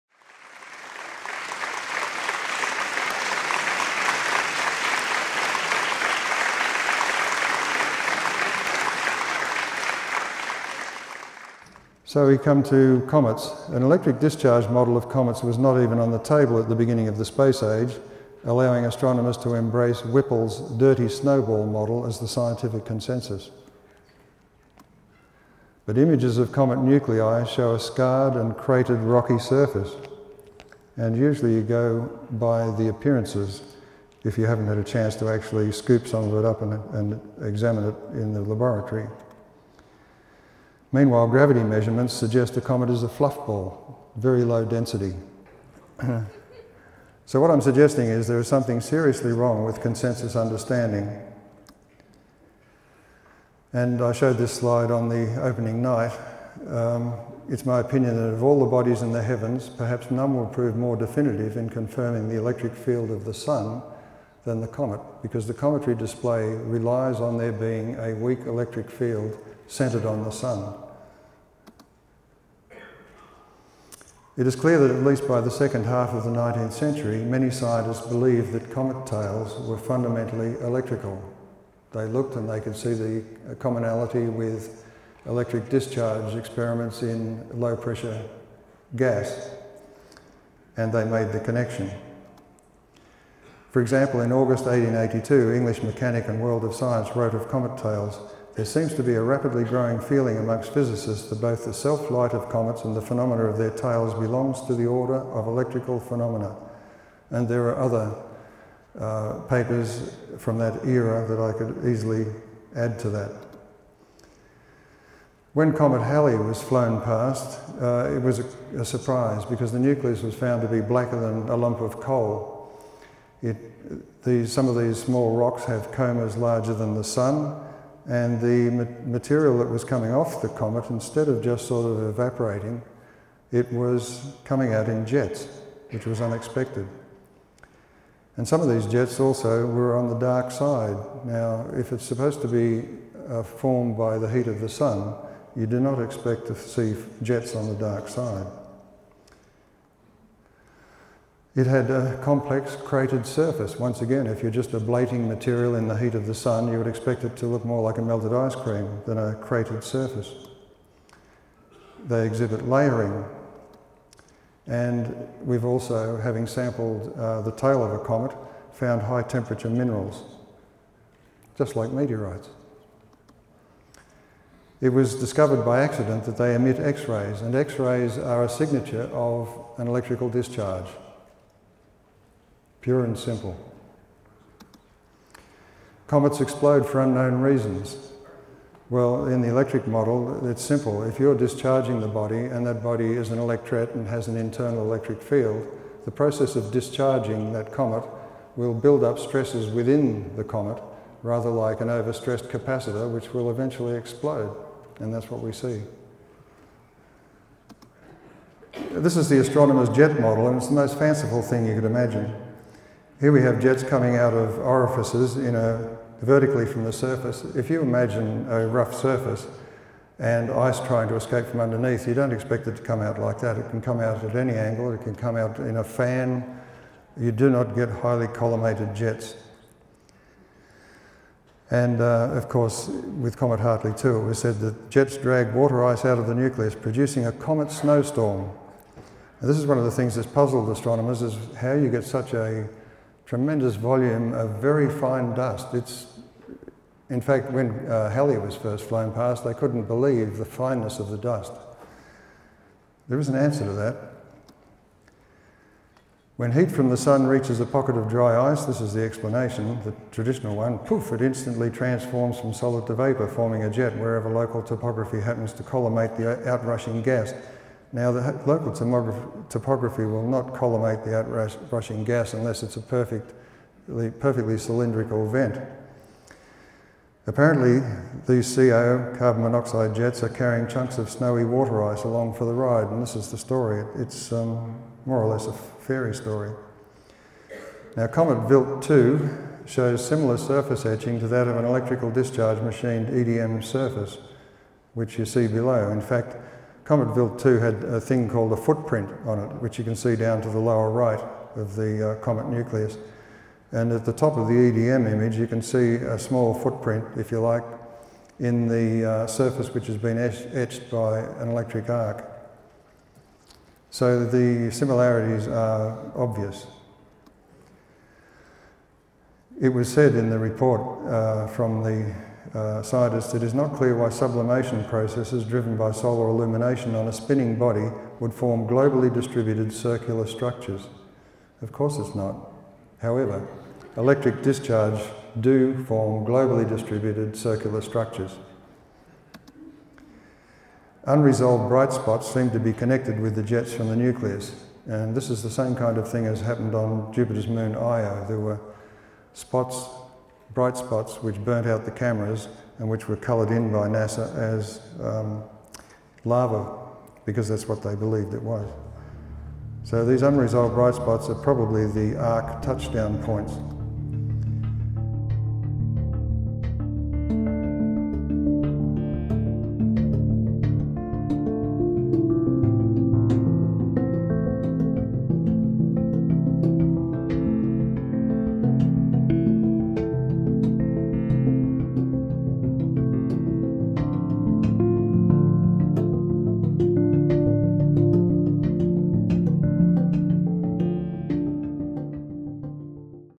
Wal Thornhill: Comets, Meteors & Asteroids | EU2012
Excerpt from Wal Thornhill's presentation at the 2012 Electric Universe Conference, The Human Story.